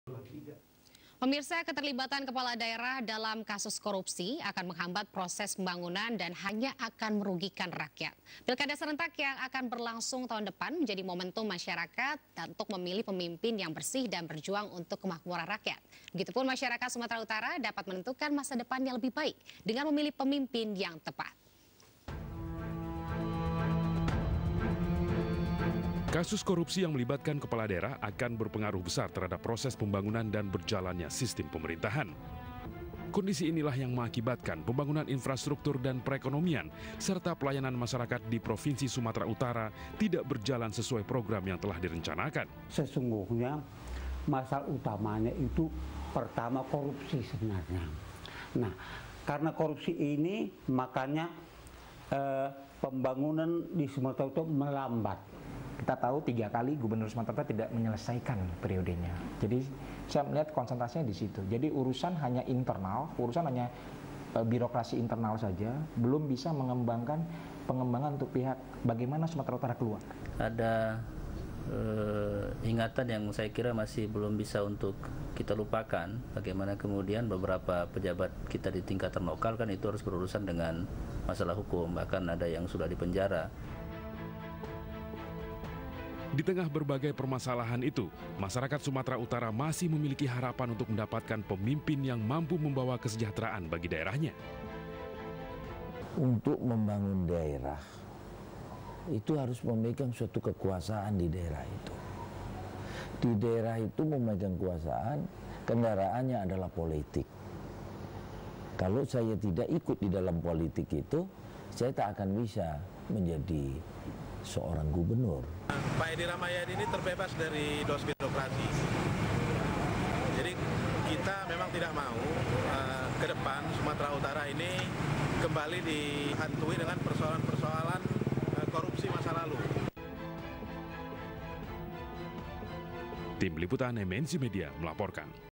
Pangkostrad Letjen Edy Rahmayadi Ingin Perbaiki Kesejahteraan Rakyat SUMUT
Keterlibatan kepala daerah dalam kasus korupsi akan menghambat proses pembangunan dan hanya akan merugikan rakyat, Pilkada serentak yang akan berlangsung tahun depan menjadi momentum masyarakat untuk memilih pemimpin yang bersih dan berjuang untuk kemakmuran rakyat, begitu pun masyarakat Sumatera Utara dapat menentukan masa depannya lebih baik dengan memilih pemimpin yang tepat